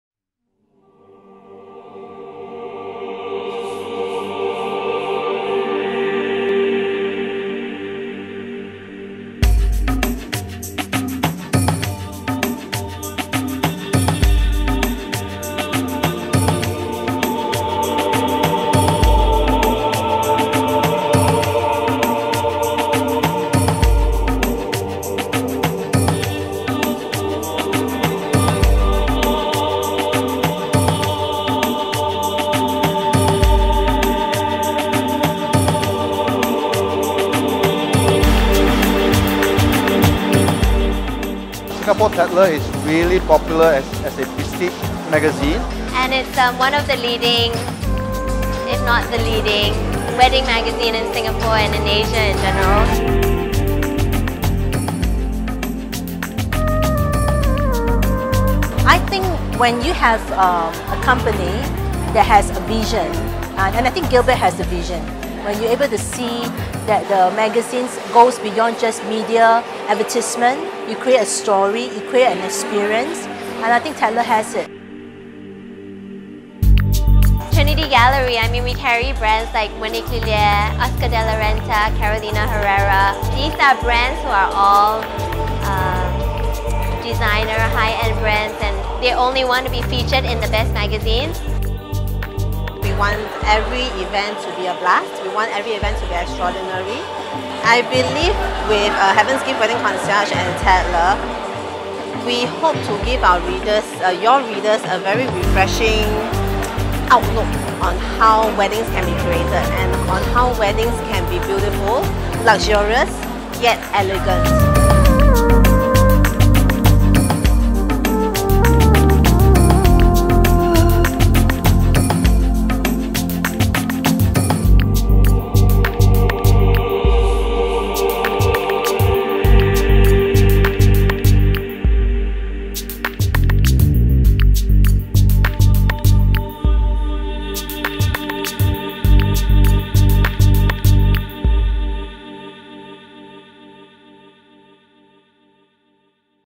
Singapore Tatler Weddings Show 2015
To unveil Singapore Tatler Weddings' Spring/Summer 2015 edition, friends and guests of the luxury wedding magazine came in full support, together with event partners Trinity Gallery, Heaven's Gift Wedding Concierge, Boenga, Bentley, Allure Weddings, One Eye Click Live, Peroni Italy and Grand Hyatt Singapore.